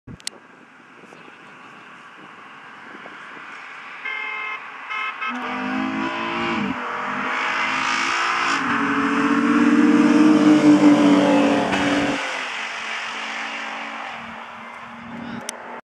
Race 2 ll Genesis Coupe 3.8 vs Aurion 3.5 Modded - HD ( in KSA )
هيونداي جينسيس vs اوريون فلتر